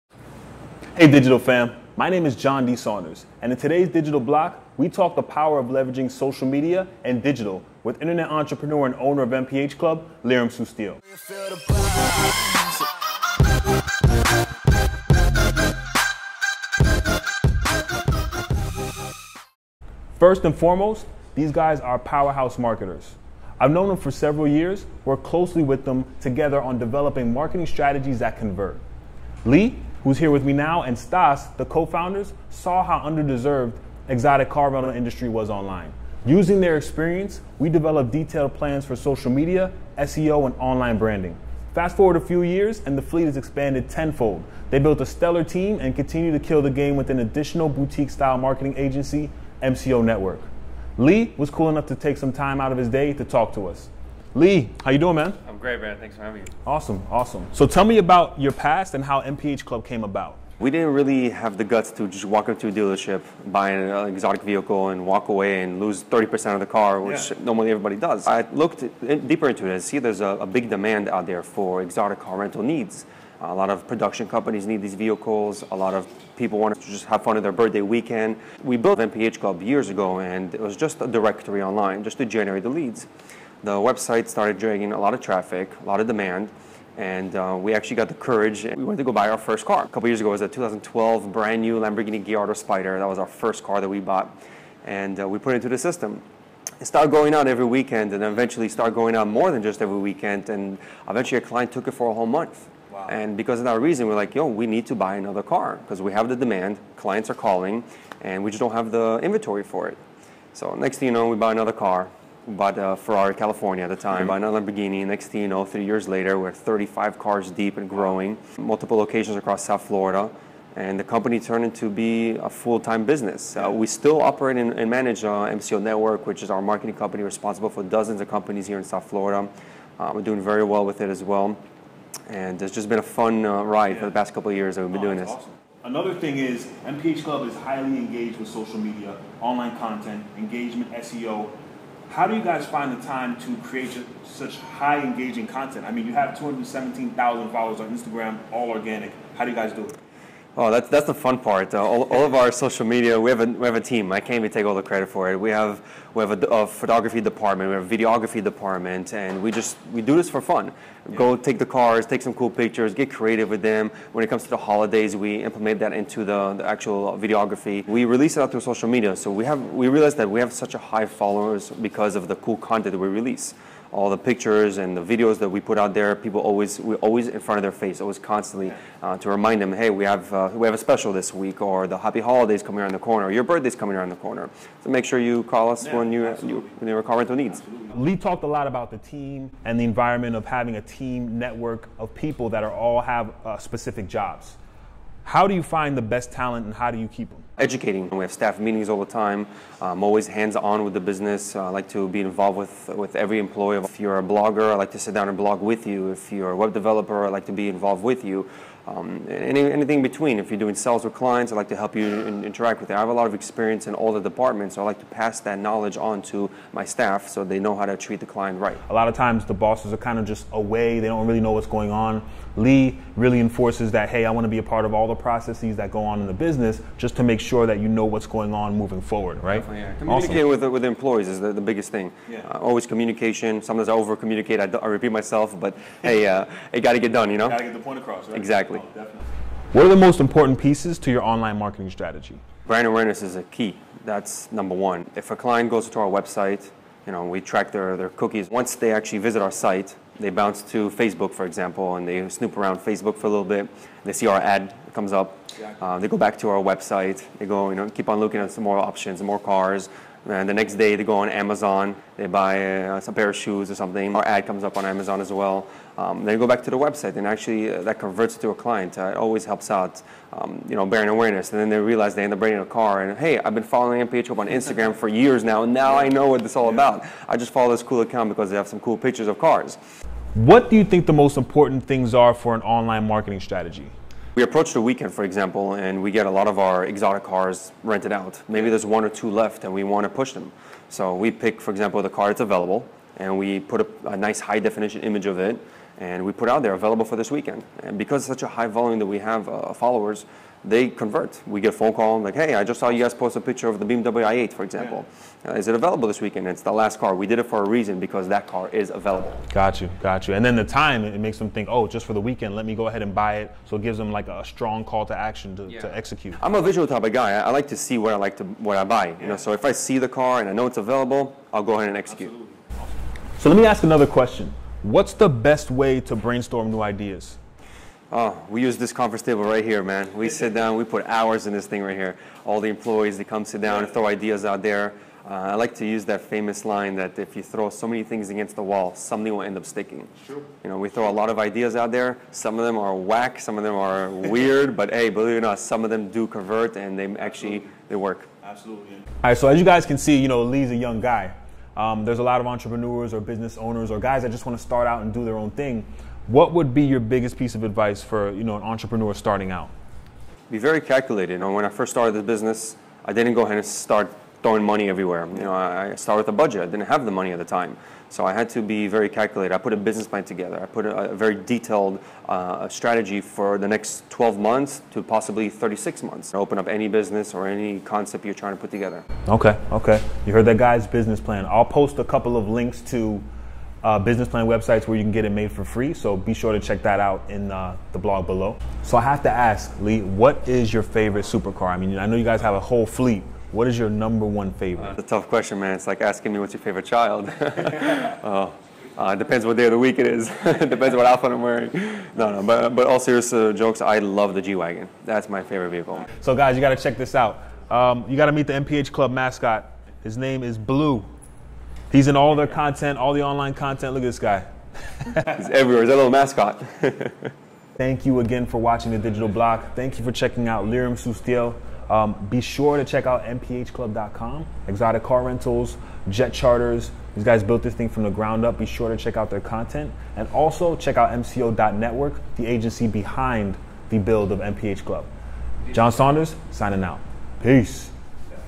Interview with Liram Sustiel | Ep. 14
Important Links

// Exotic Car Rentals in Miami

// MCO.Network

// Free Business Plan

Hey Digital Fam, my name is John D. Saunders, and in today’s Digital Block we talk the power of leveraging social media and Digital with Internet Entrepreneur and owner of mph club®, Liram Sustiel.

First and foremost, these guys are powerhouse marketers. I’ve known them for several years, worked closely with them, together, on developing marketing strategies that convert. Lee, who’s here with me now, and Stac, the co-founder, saw how under deserved exotic car rental industry was online. Using their experience, we developed detailed plans for social media, SEO and online branding. Fast forward a few years and the fleet has expanded 10 fold. They’ve built a stellar team and continue to kill the game with an additional boutique style marketing agency, MCO Network. Lee was cool enough to take some time out of his day to talk to us.

John: So, tell me about your past and how mph club came about.

Lee: We didn't really have the guts to just walk into dealership, buy an exotic vehicle and walk away and lose 30% of the car which normally, everybody does. I've looked deeper and see there's a big demand out there for exotic car rental needs. A lot of production companies need these vehicles. A lot of people wanted to just have fun in their birthday weekend.

We built mph club years ago and it was just a directory online, just to generate the leads. The website started generating a lot of traffic, lot of demand and we actually got the courage and went to go buy our first car a couple of years ago. It was a 2012 brand new Lamborghini Gallardo Spyder. That was the first car that we bought and we put it into the system. It started going on every weekend and eventually started going out more than just every weekend and eventually, our client took it for a whole month.

And because of that reason, we're like, yo, we need to buy another car because we have the demand, clients are calling and we just don't have the inventory for it. So the next thing we know, we buy another car, buy the Ferrari California at that time, buy another Lamborghini and the next thing you know, three years later, we're 35 cars deep and growing, multiple occasions across South Florida and the company turned to be a full time business. So, we still operate and manage on MCO Network which is our marketing company responsible for dozens of companies here in South Florida. We're doing very well with it as well and it's just been a fun ride for the last couple of years we've been doing this.

John: Another thing is mph club is highly engaged with social media, online content, engagement SEO. How do you guys find the time to create such high engaging content? I mean, you have a total of 17,000 followers on Instagram, all organic. How do you guys do it?

Lee: That's the fun part. My team can take all the credit for it. We have a photography department. We have a videography department and we do this for fun. We go take the car, take some cool pictures, get creative with them. When it comes to the holidays, we implement that into the actual videography.

John: What are the most important pieces to your online marketing strategy?